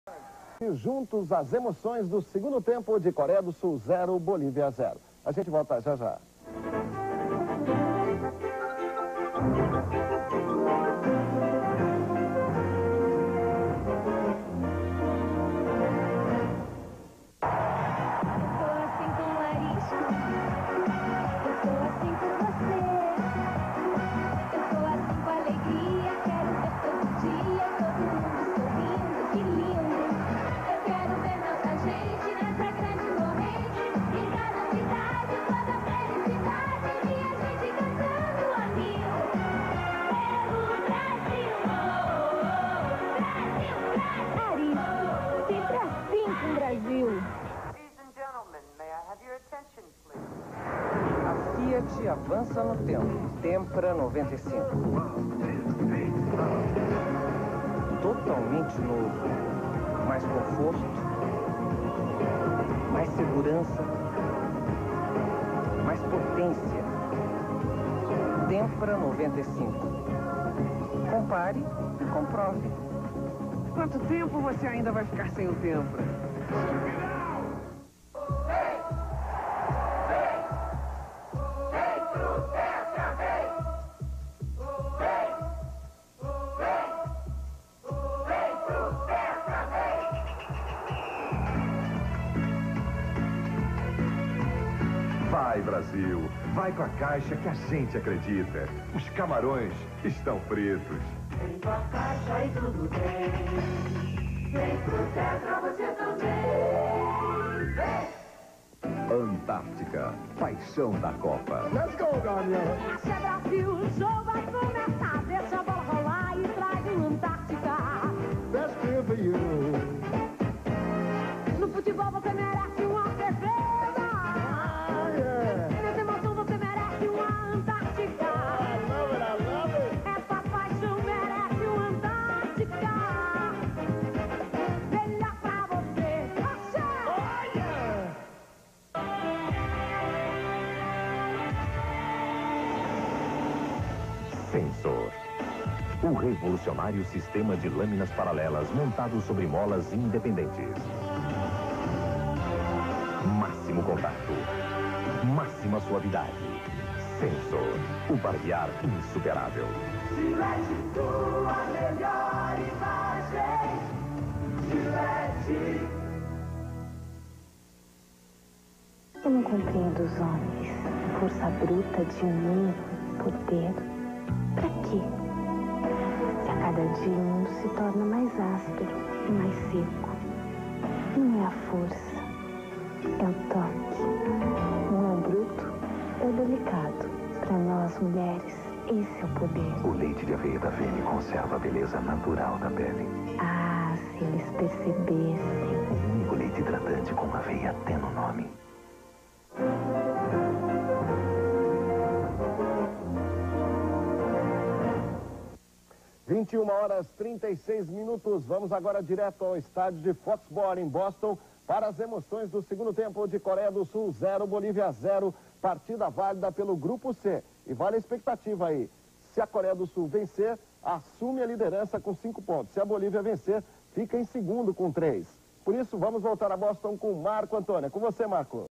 Intervalo Copa 94 Band (23/06/1994) [2/3]
Acessa Blog RHB Na TV
Intervalo de comercial da copa 94 na band Em 23 de Junho de 1994 bloco 2 pelo exibição na band 1994